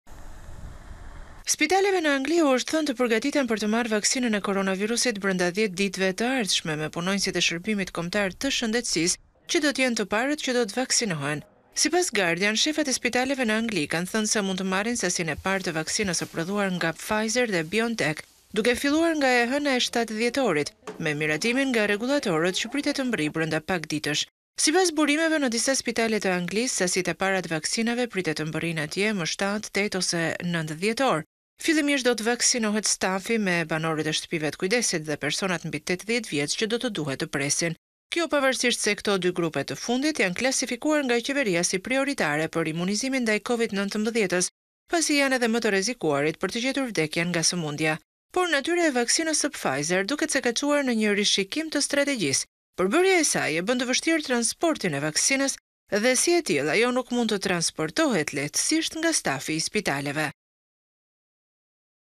Top News - Spitalet në Angli/ Vaksina antiCovid vjen për 10 ditë
Spitaleve në Angli u është thënë të përgatiten për të marrë vaksinën e koronavirusit brenda 10 ditëve të ardhshme, me punonjësit e shërbimit kombëtar të shëndetësisë që do të jenë të parët që do të vaksinohen.

Sipas Guardian, shefat e spitaleve në Angli kanë thënë se mund të marrin sasinë e parë të vaksinës së prodhuar nga Pfizer dhe Biontech duke filluar nga e hëna e 7 dhjetorit, me miratimin nga rregullatorët që pritet të mbërrijë brenda pak ditësh. Sipas burimeve në disa spitale të Anglisë, sasitë e para të vaksinave pritet të mbërrijnë atje më 7.8 ose 9 dhjetor.

Fillimisht do të vaksinohet stafi me banorët e shtëpive të kujdesit dhe personat mbi 80 vjeç që duhet të presin. Kjo, pavarësisht se këto dy grupe janë klasifikuar nga qeveria si prioritare për imunizimin ndaj Covid-19, pasi janë edhe më të rrezikuarit të gjejnë vdekjen nga sëmundja. Por, natyra e vaksinës së Pfizer-it duket se ka çuar në një rishikim të strategjisë. Përbërja e saj e bën të vështirë transportin e vaksinës dhe si e tillë ajo nuk mund të transportohet lehtësisht nga stafi i spitaleve.

Pamje: AFP